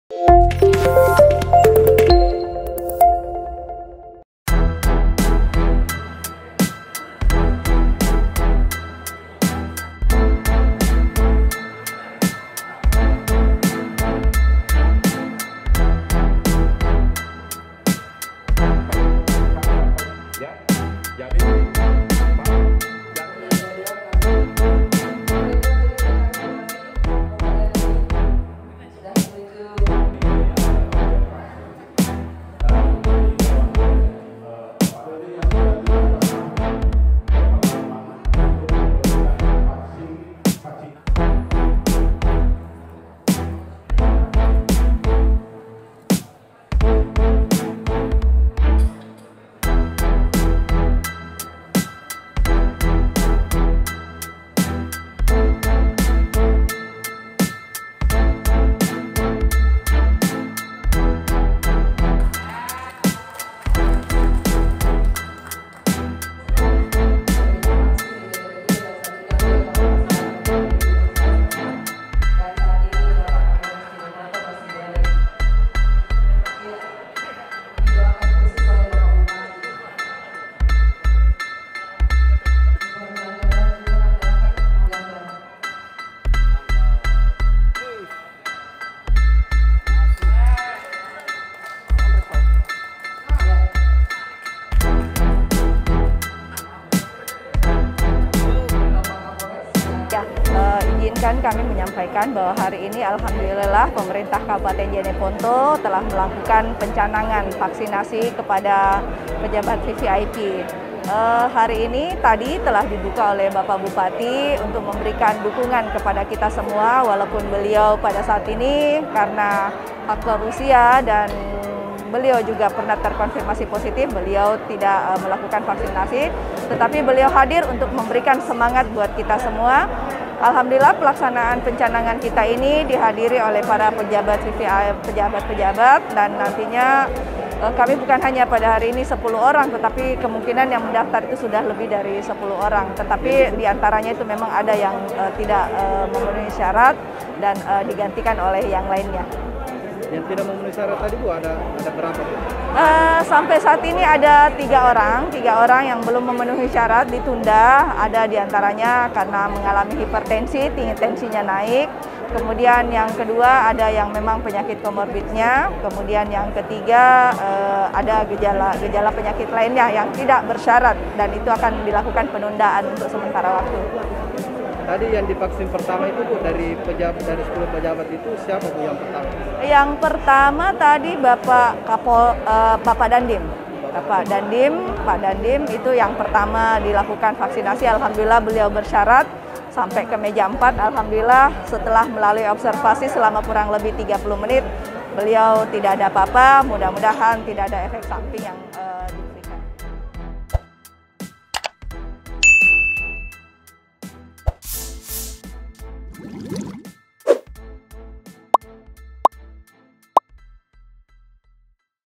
Pemkab Jeneponto Gelar Vaksinisasi
Pemerintah Kabupaten Jeneponto menggelar pencanangan Vaksinisasi Covid-19 pertama di aula Kantor Bupati Jeneponto, Jl. Lanto dg Pasewang, Senin (1/2/2021).

Dalam proses Vaksinisasi,
Bupati Jeneponto Iksan Iskandar dan Sekda Jeneponto dan beberapa pimpinan OPD hadir di tempat tersebut, namun mereka tak divaksin lantaran sudah terkonfirmasi positif Covid-19.

Kepala Dinas Kesehatan Kabupaten Jeneponto, Susanti kepada sulselsatu.com mengatakan, Vaksinisasi ini di lakukan untuk 10 pejabat VIP yang telah mendaftarkan diri dan belum pernah terkonfirmasi positif Covid-19.

Video terkini dan terbaru..